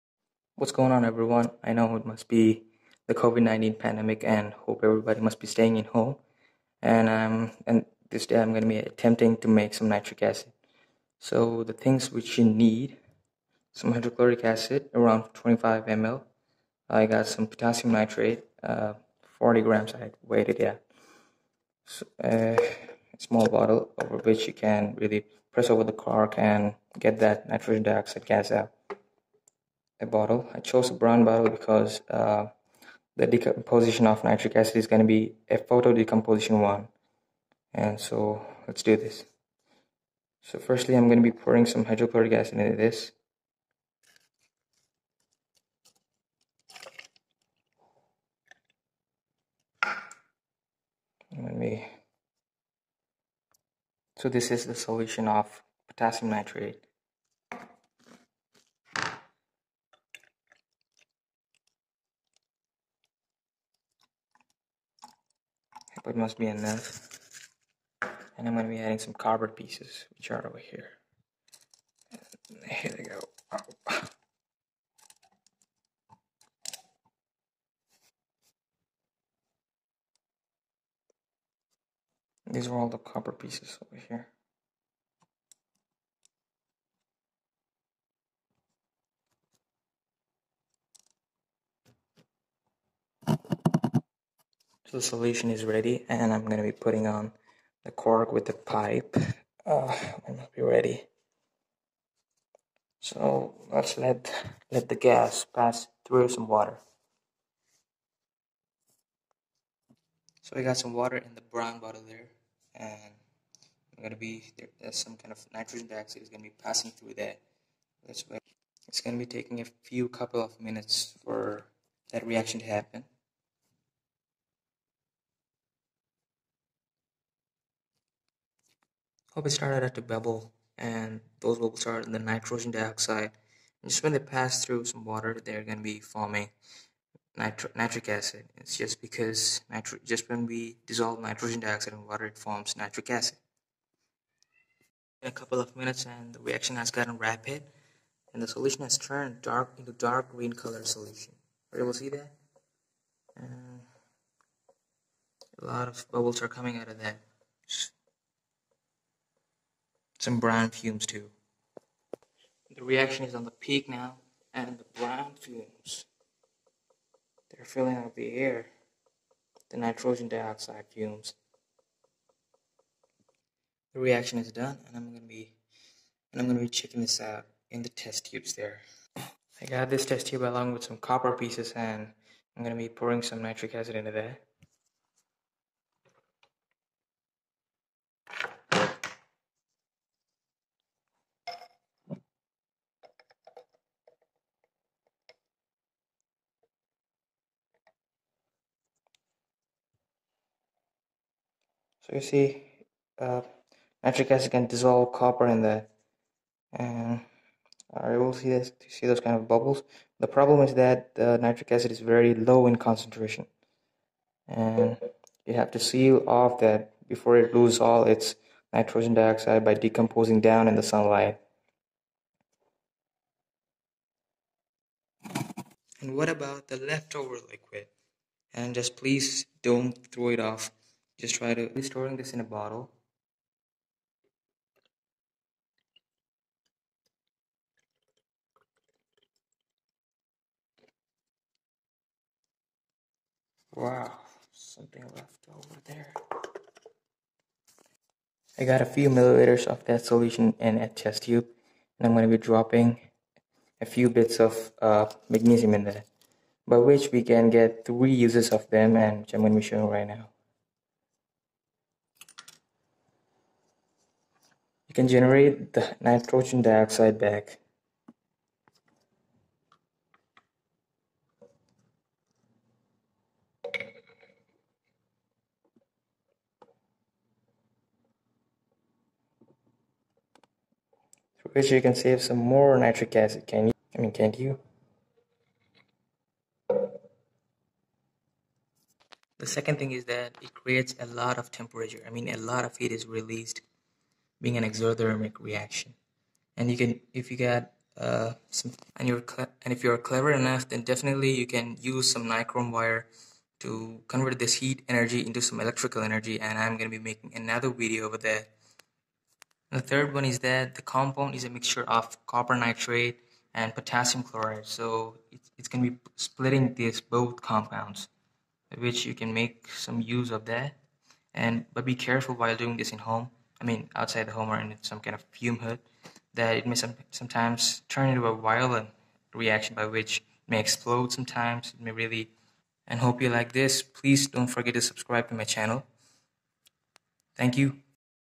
How to make nitric acid with simple stuff at home.
Hello everyone, got pretty bored staying in home and have tried to make some nitric acid. And if you want to do this make sure to do this in a fume hood arena. I did this in a room with its four windows open and a door too.

how to make nitric acid at home with basic stuff